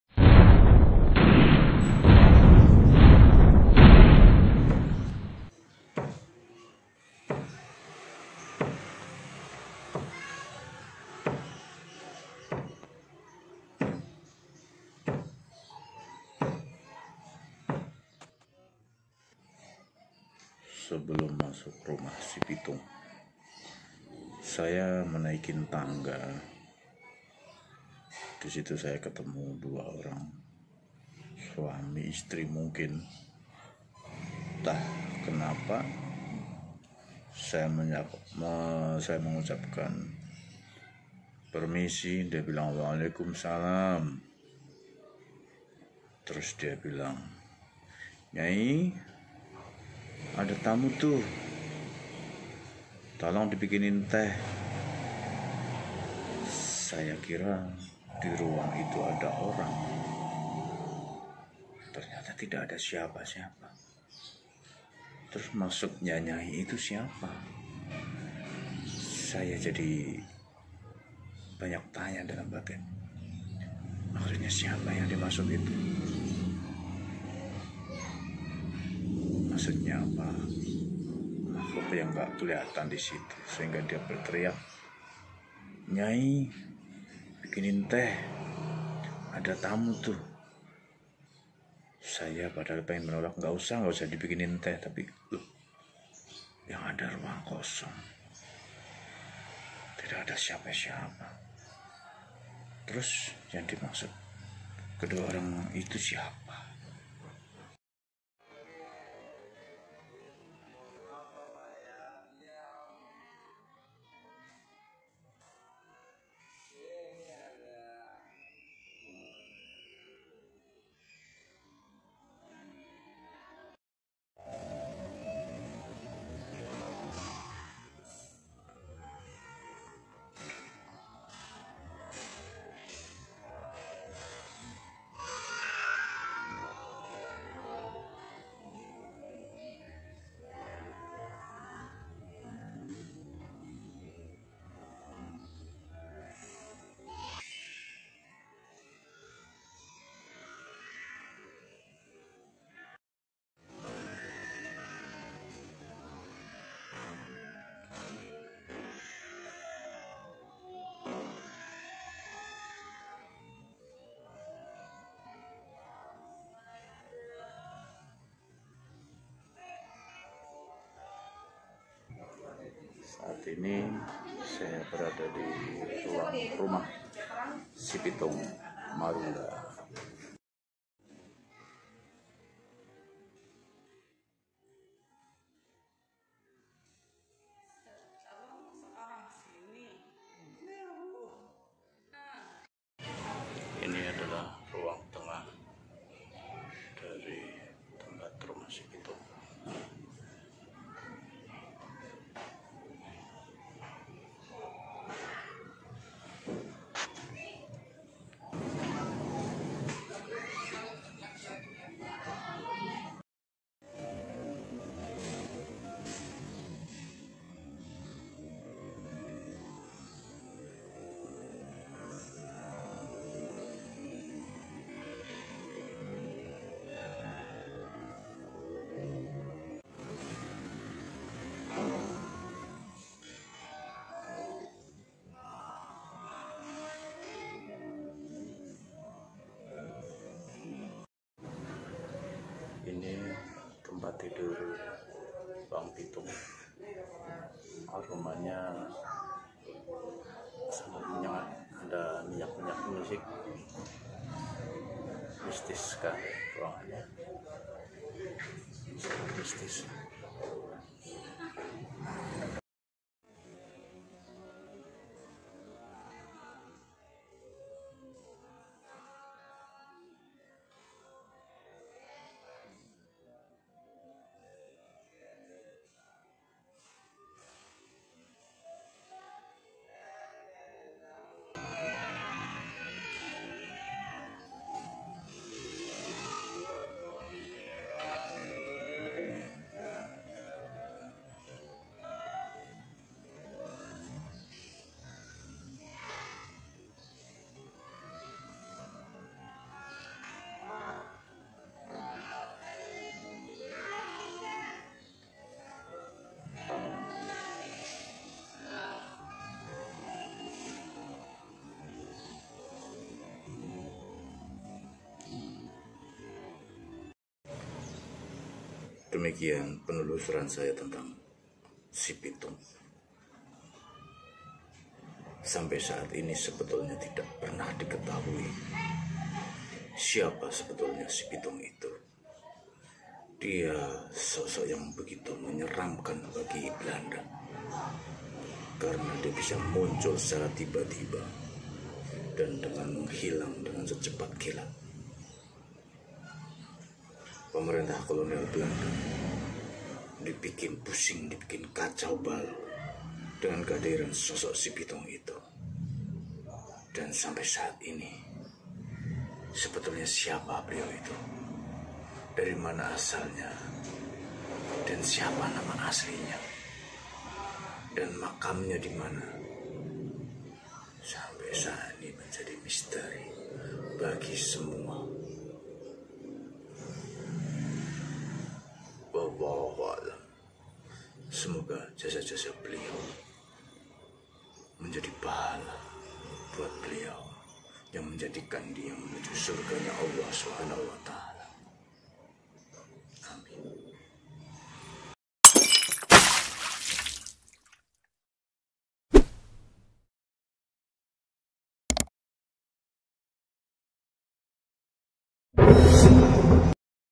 Misteri rumah si Pitung
Si Pitung sosok misteri yang tidak terungkap sampai saat ini...rumah sipitung Marunda sebagai bukti keberadaan si pitung waktu itu